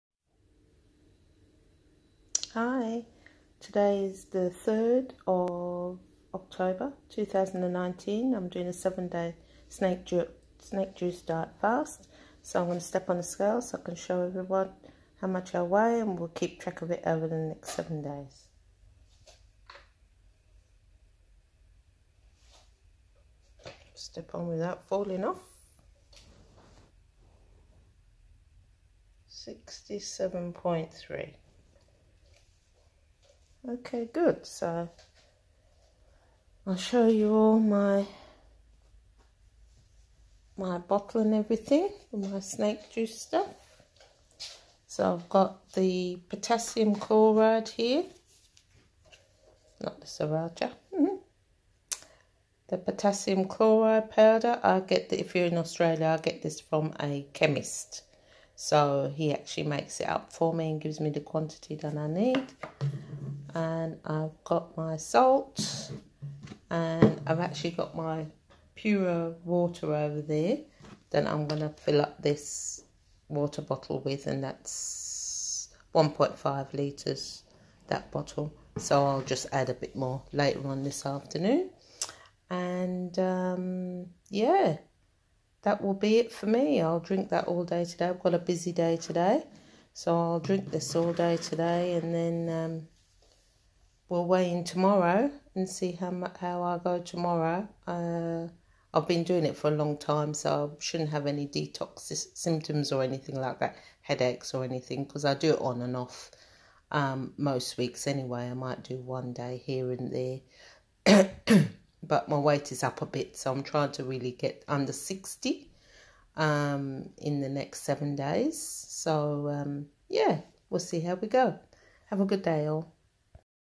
Snake juice fast/ Day 1 weight in.
1 tsp of potassium chloride +1 tsp Himalayan salt to 2ltrs water. Drink and be merry.
Feel good will do 30 minutes walk today.
30 minutes snake juice sauna.